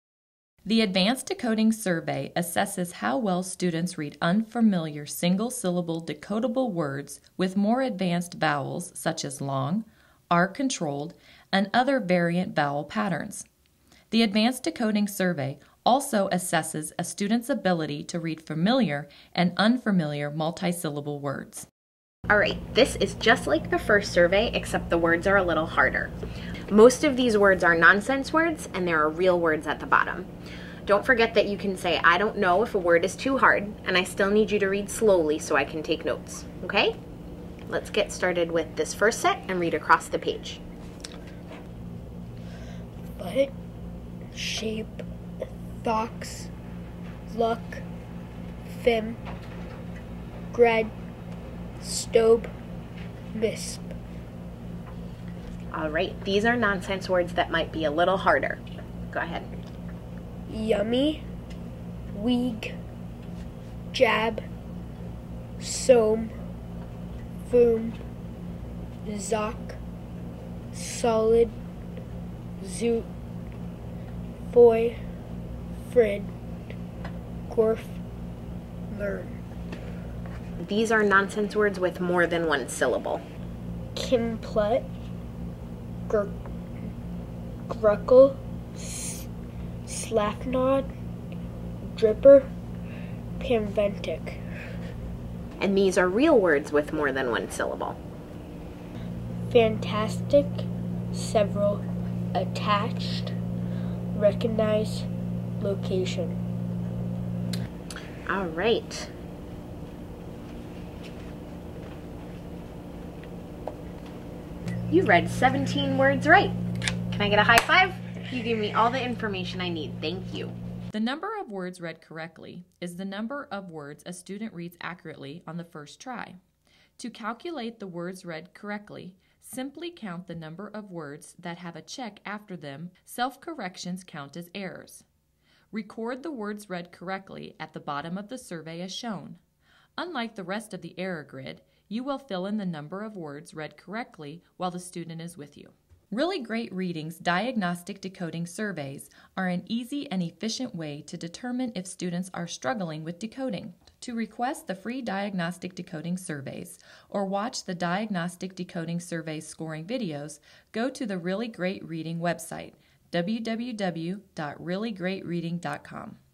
Advanced Decoding Survey: Administration
Educators using the surveys can identify which basic reading skills have been mastered and which skills need improvement. The Advanced Decoding Survey assesses how well students read unfamiliar single-syllable decodable words with more advanced vowels such as long, r-controlled, and other variant vowel patterns. The Advanced Decoding Survey also assesses a student's ability to read unfamiliar and familiar multi-syllable words.